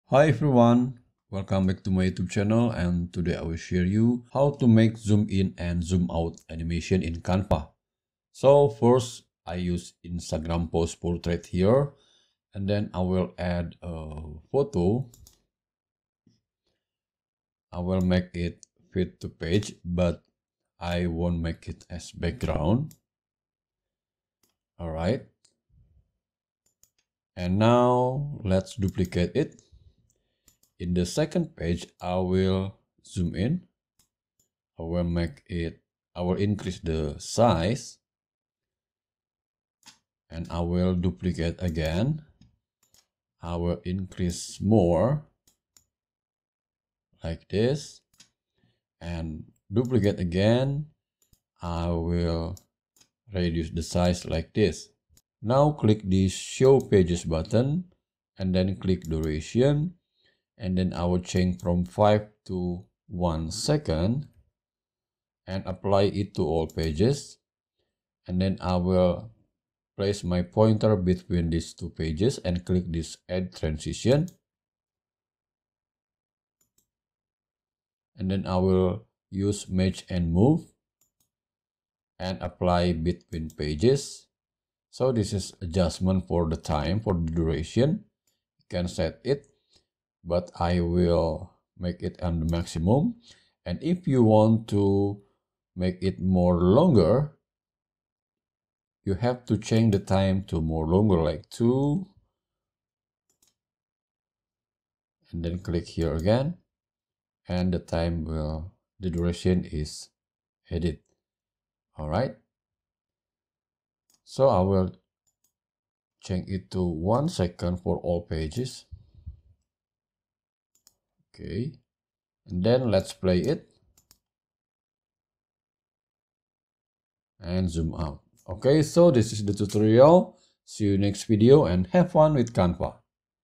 BEGINNERS do this: ZOOM In and Out Animation Canva Tutorial
In this Canva Animation tutorial for beginners I will share you a basic motion graphics trick: how to use Canva to make a zom in and zoom out animation.